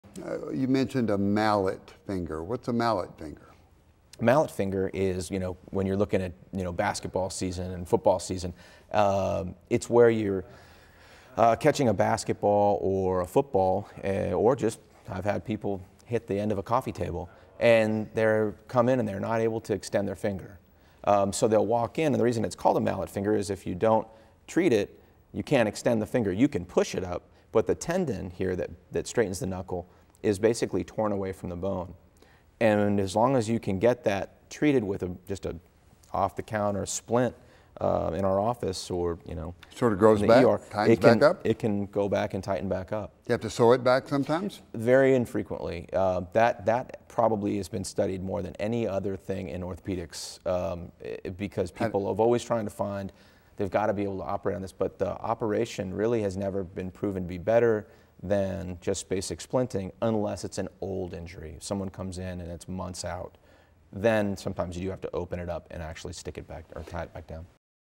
Mallet Finger: How Is It Treated?
Mallet finger, also known as dropped finger and baseball finger, is when injury occurs to the extensor tendon, preventing the finger to fully straighten. Orthopedic surgeon Dr. Tim Renfree describes the symptoms and treatments for this common sports injury.